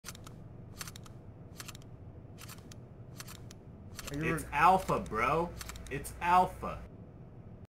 "Its ALPHA bro, its ALPHA!"
Yea. Later on the AR stopped shooting at 14 bullets and got me killed.